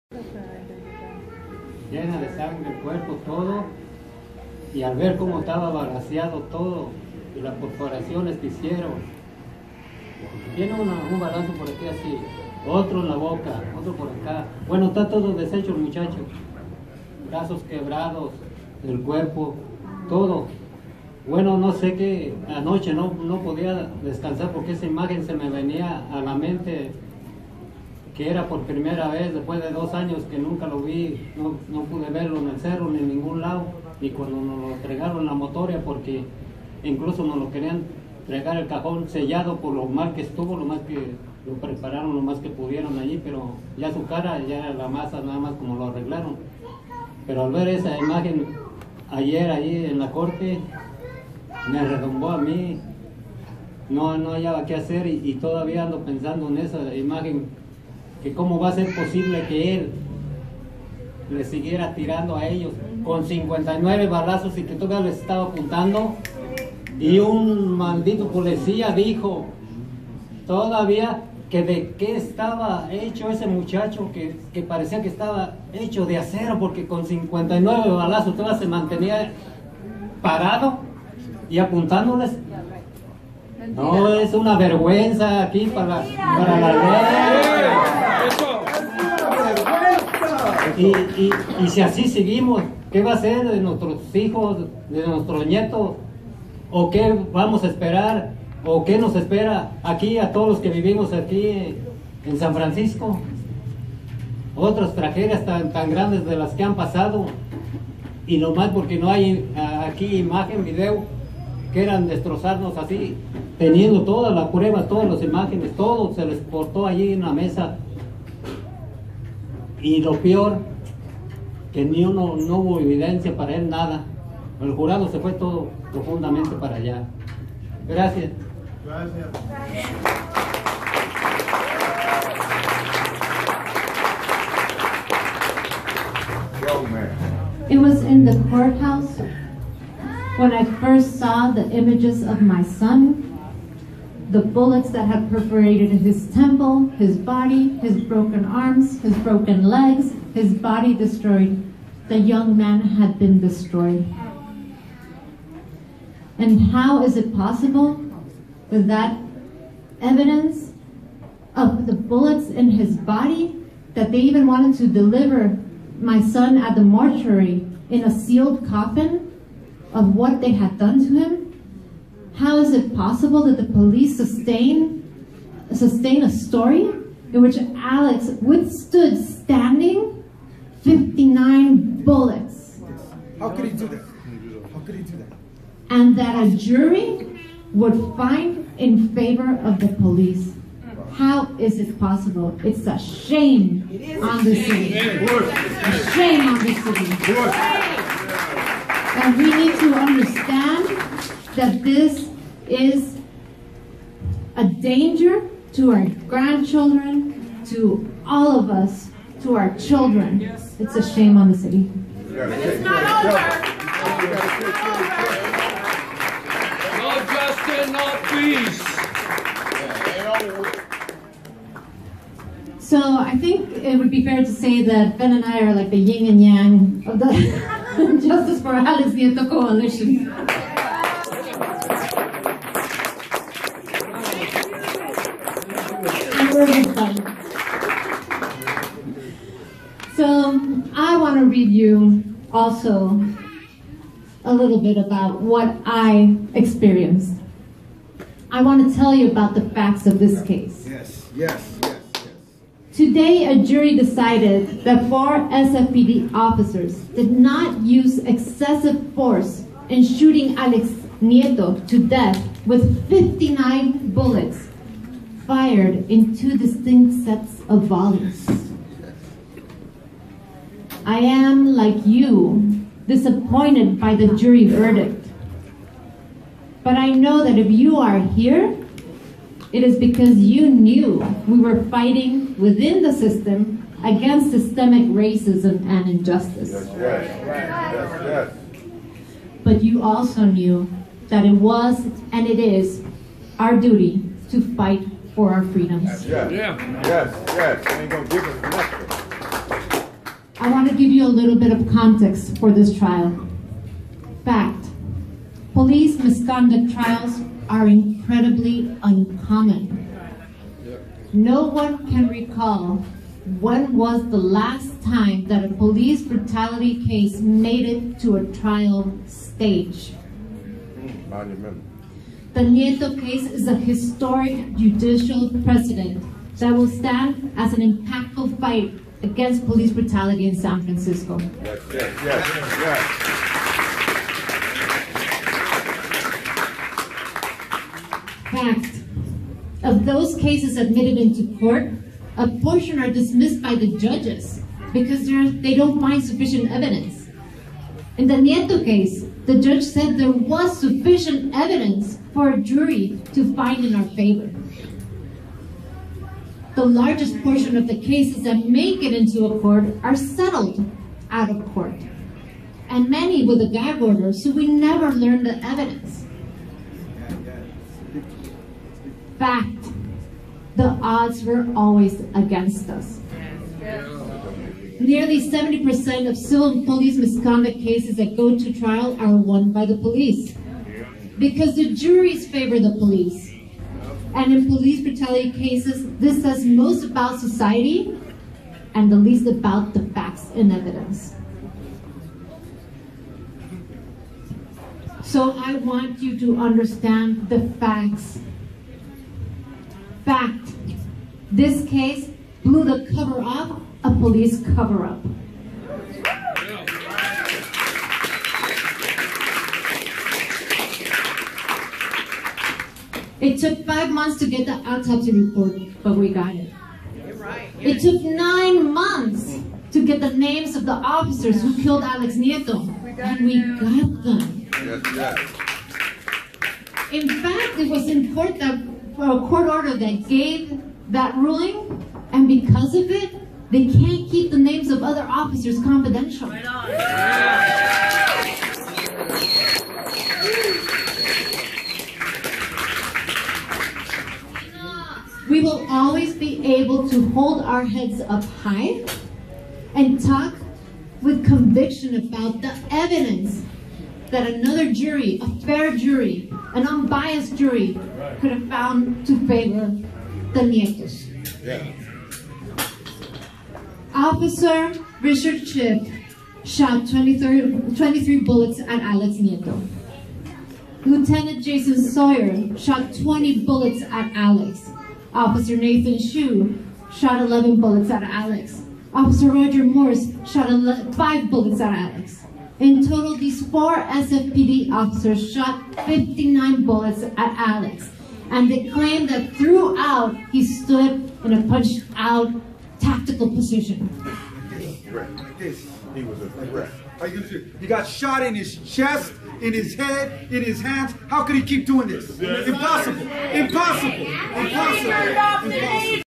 Refugio Nieto (Alex's father) speaks following jury verdict to let police away with murder
Refugio speaks in Spanish, Adrianna later translates